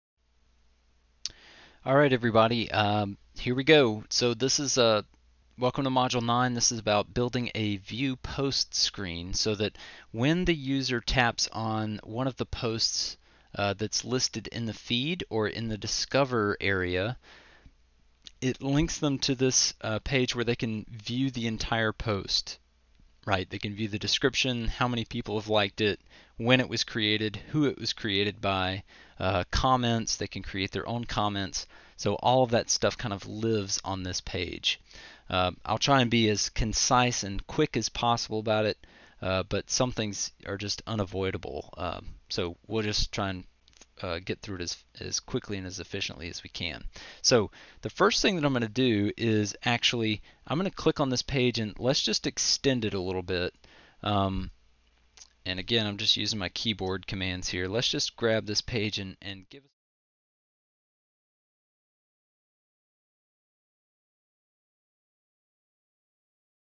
Learn to build an Instagram clone in Adalo - Module 9 Preview
You’ll build an Instagram clone with photos upload, profile pages,
commenting, messaging, notifications, and search features.

Course Skills:
• Build databases that are simple and well-labeled
• Put the user experience first
• Recognize the affordances and limitations of Adalo
• Practice design and layout skills
• Create working links between databases and visual elements
• Show the benefits of actions, visibility rules, and filtering.

Module 9 Objective:
Create a screen for users to see the full post and comment on it.
• Layout and link the elements
• Add a list of comments
• Add a comment input field